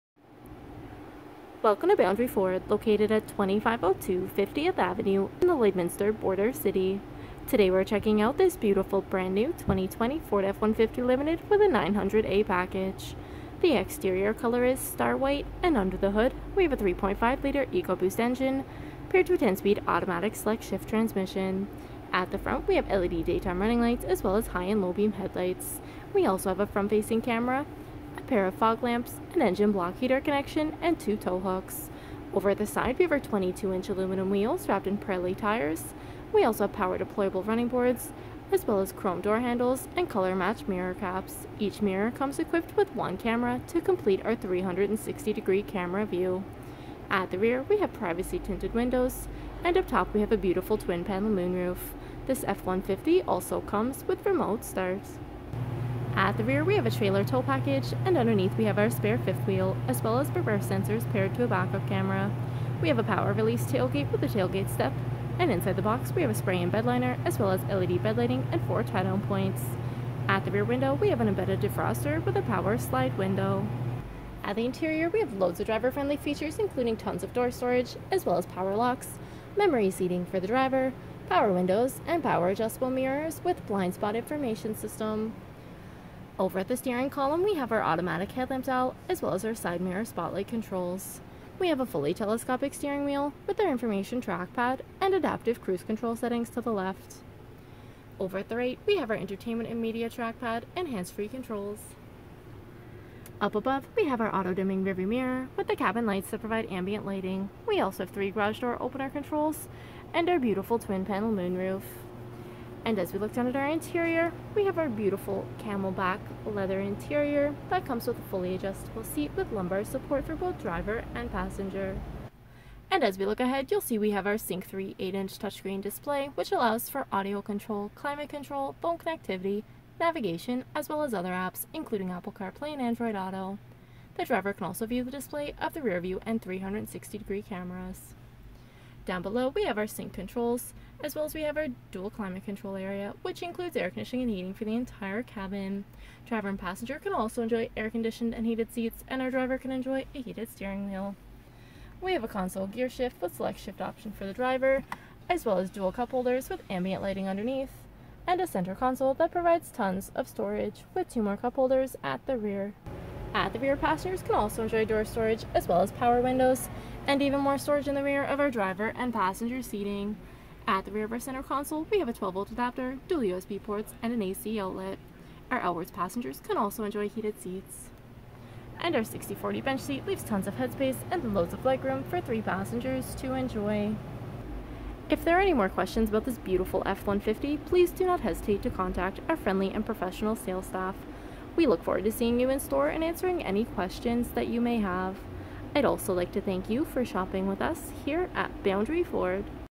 2020 Ford F-150 Limited 900A W/ 3.5L EcoBoost, 360 Camera, Pano Roof Overview | Boundary Ford 20T075
The 2020 Ford F-150 Limited is exactly what you've been looking for! Equipped with a 3.5L EcoBoost engine paired to a 10 speed automatic transmission and electronic shifting 4 wheel drive, there's no where this truck can't take you! The beautiful Star White exterior is complimented with chrome grille, door handles and 22 inch aluminum wheels. The look is completed with the Limited Tailgate. This truck is loaded with features to impress before you even climb inside including power deployable running boards, power release tailgate with a tailgate step, spray-in bedliner, LED box lighting, 360 degree camera and reverse sensors paired to a backup camera. The incredible features are carried to the interior and include beautiful leather seating with heating and cooling options for front passengers, and heating options for rear passengers, heated steering wheel, a beautiful twin panel moonroof, voice activated navigation, adaptive cruise control and SO much more! The features are endless, so stop in today to experience everything the 2020 F-150 Limited has to offer! Here at Boundary Ford we're prairie proud and we offer the best deals in the prairies!
20T075